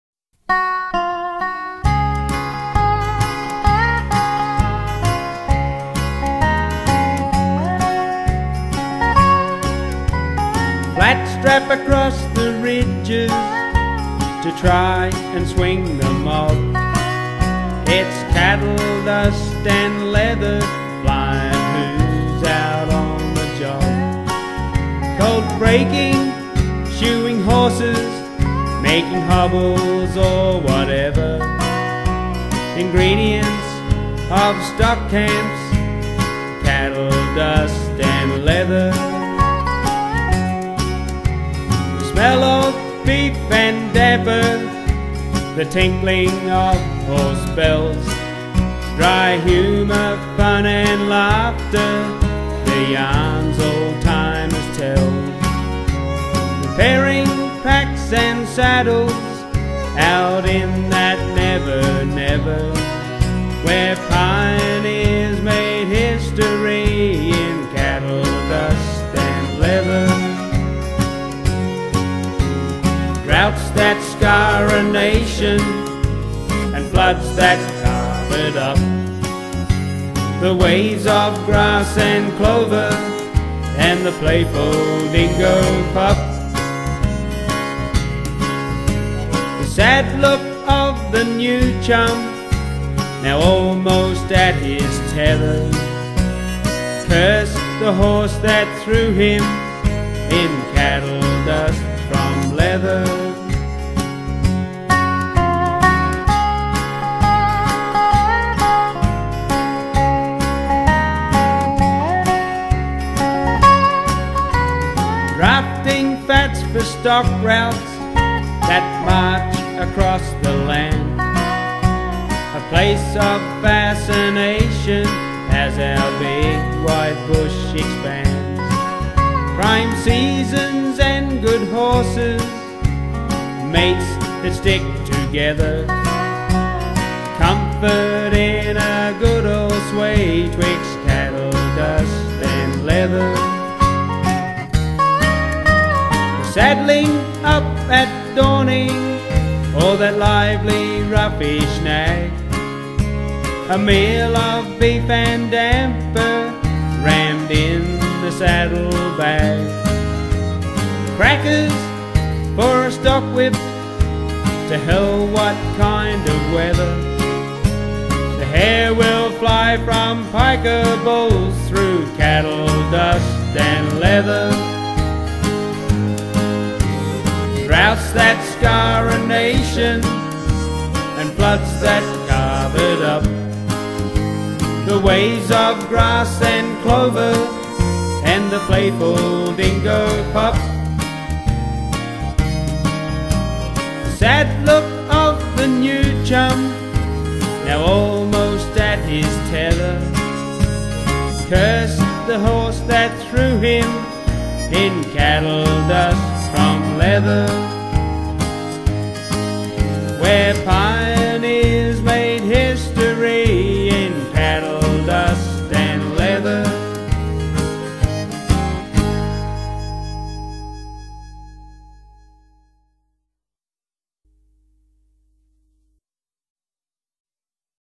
"Cattle Dust & Leather"
Bush poem written by Joe Daley and brought to life by Ashley.